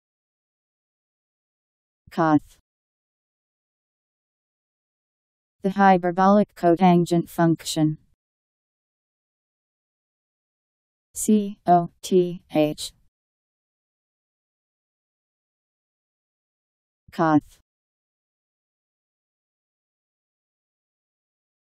Coth Meaning
Video shows what coth means. The hyberbolic cotangent function..  Coth Meaning. How to pronounce, definition audio dictionary. How to say coth. Powered by MaryTTS, Wiktionary